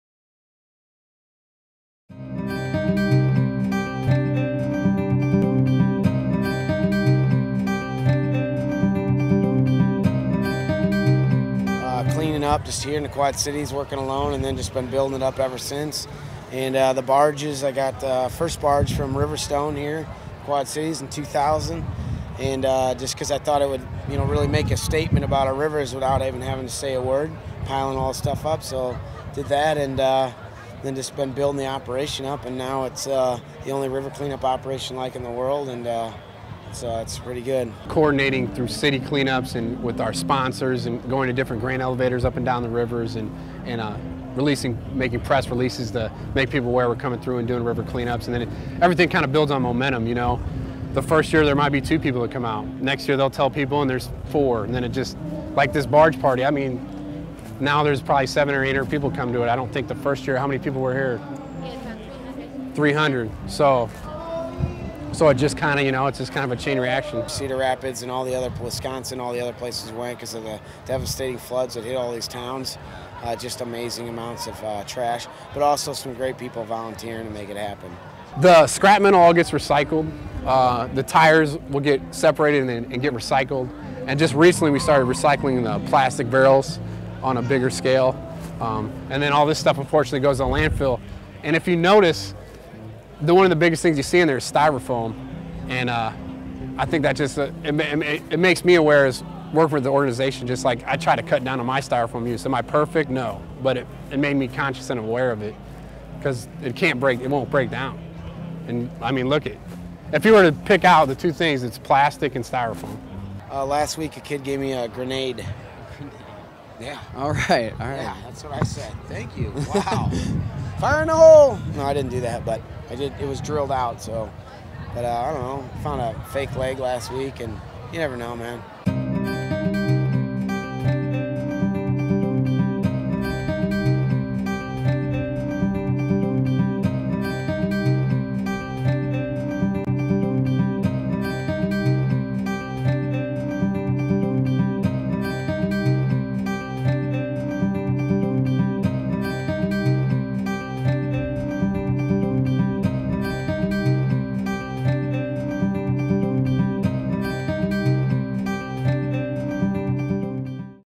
Living Lands & Water.mov
Cleaning up the Mississippi River one piece of trash at a time.